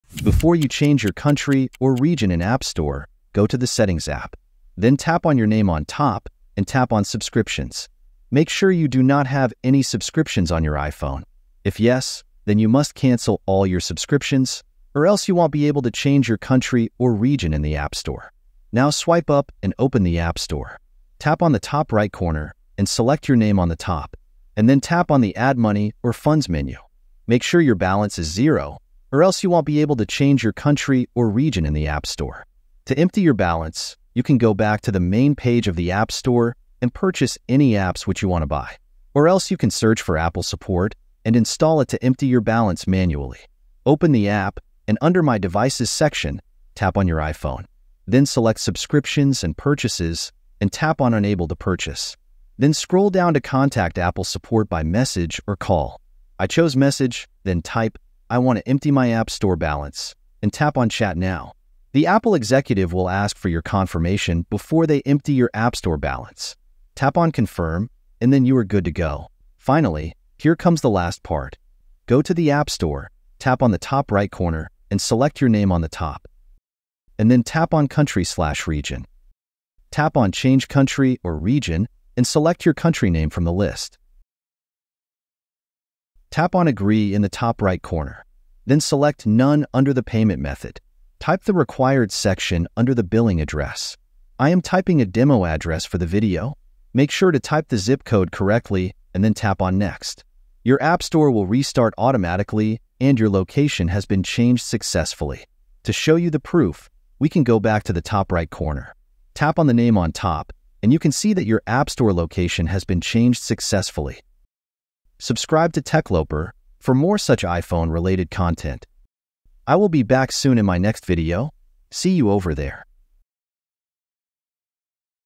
How to Change Region in App Store? Change App Store Country on iPhone 15 Plus
How to Change region in app store? Change Appstore Country on iphone 15 plus from the United States to India

Follow the step by step guide in the video so you do not make any mistakes when you decide to change the country or region in app store. This method works on both ios 17 and ios 18 installed on your iphone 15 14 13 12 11.

0:00 subscription setting
0:18 app store balance setting
0:43 apple support app setting
1:11 change country/region in appstore
1:55 more iphone tech perks